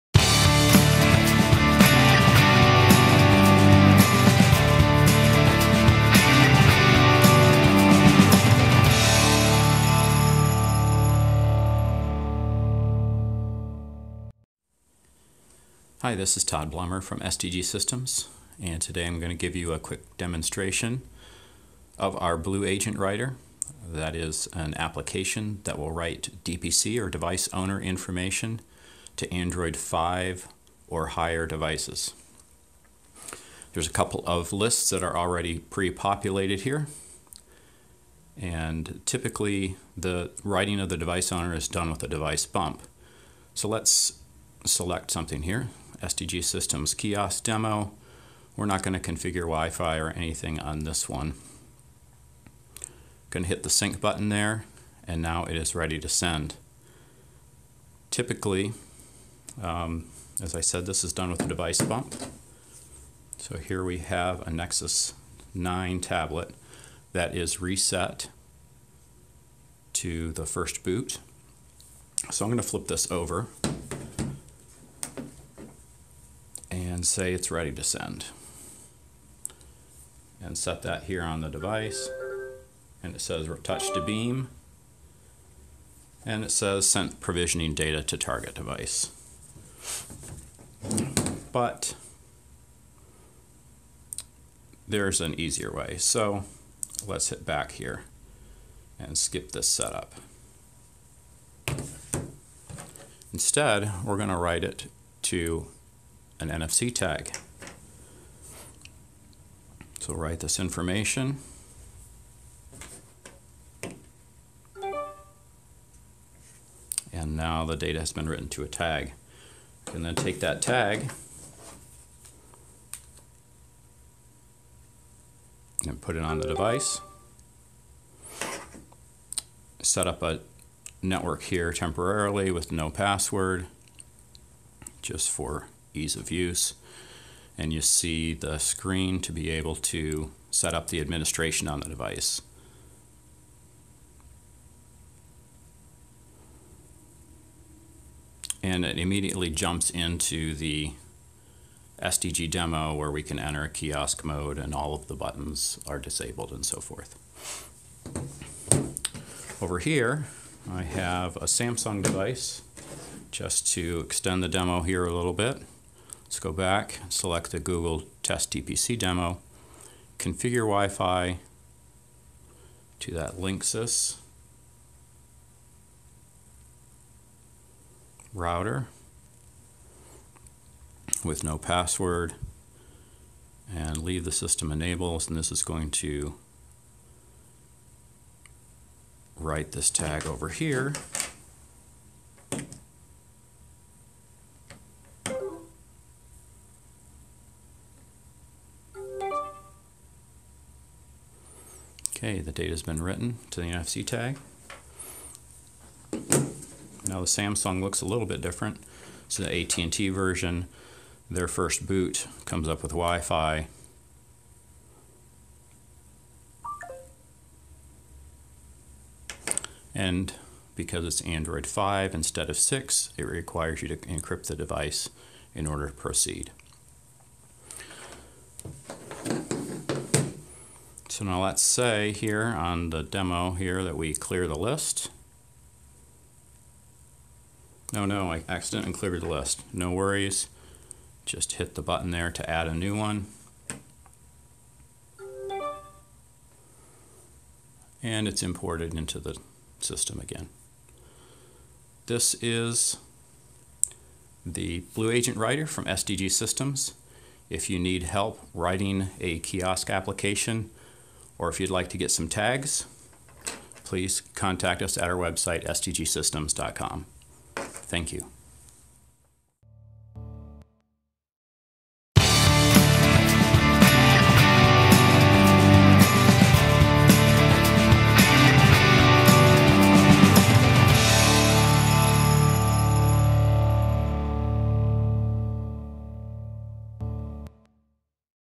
Android Device Owner / DPC / Kiosk write to NFC tag
This demo shows how our Blue Agent Writer app will write the Android device owner or DPC (device "agent") information to a tag that may then be used to provision devices. Our Blue Agent Writer is available in the Google Play store (free)